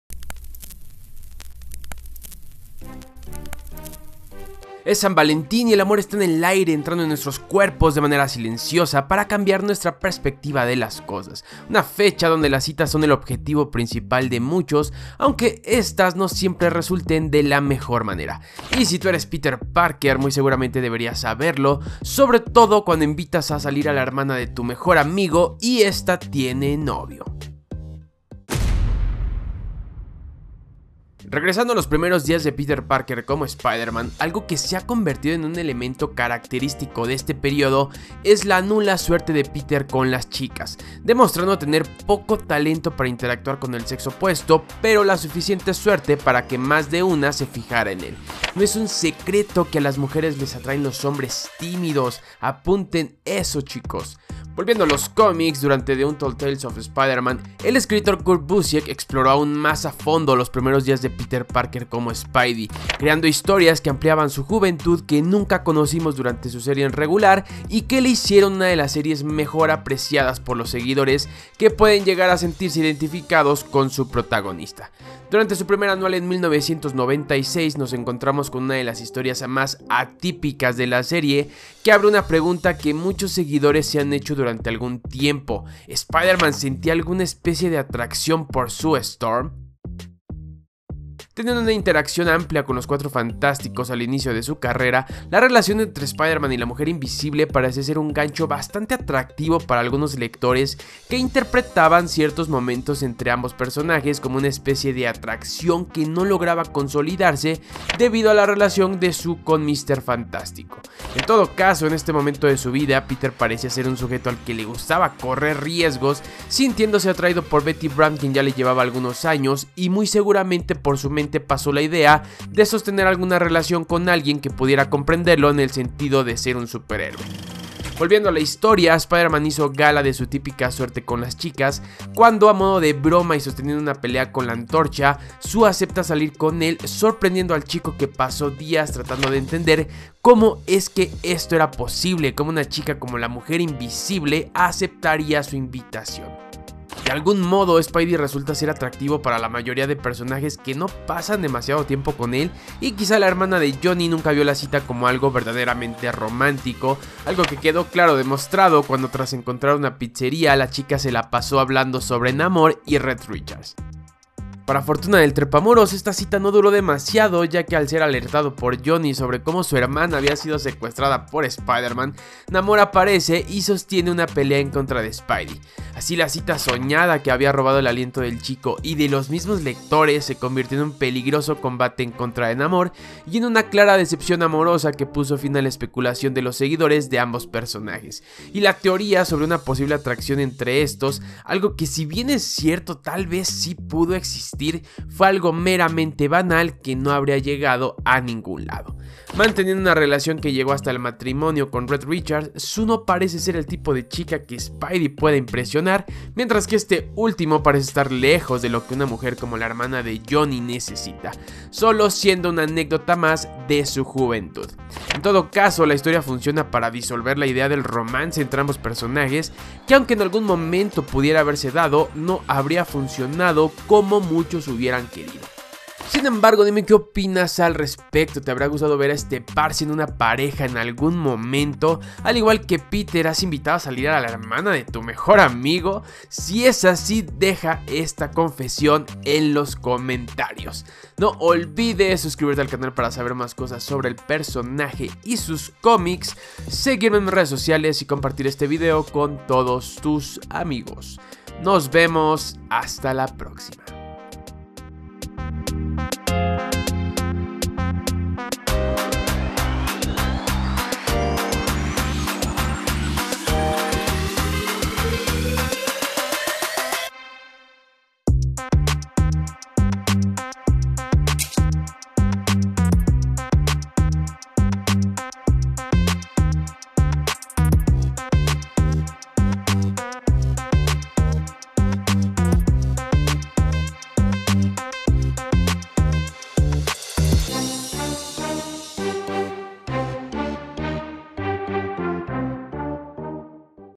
El peligroso ROMANCE entre Spider-Man y la Mujer Invisible
Es San Valentin y el amor está en el aire, entrando en nuestros cuerpos de manera silenciosa para cambiar nuestra perspectiva de las cosas. Una fecha donde las citas son el objetivo principal de muchos, aunque estas no siempre resultan de la mejor manera.

Y si eres Peter Parker, muy seguramente deberías saberlo, sobre todo cuando cuando invitas a salir a la hermana de tu mejor amigo y esta tiene novio.